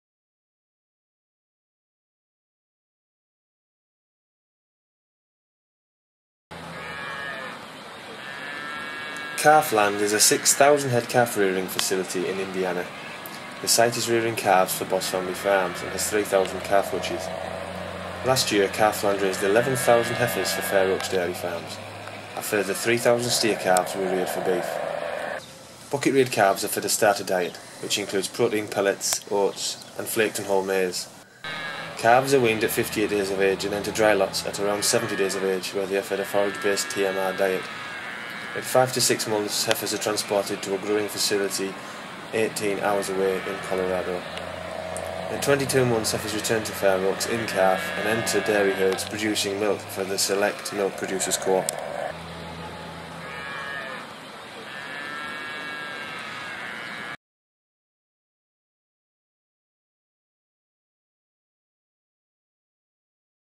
Calf Land Indiana, USA
Farmers Weekly visits a 6,000-head calf-rearing facility in Indiana, USA to find out how the farm operates and how calves are reared in the 3,000 hutches on site.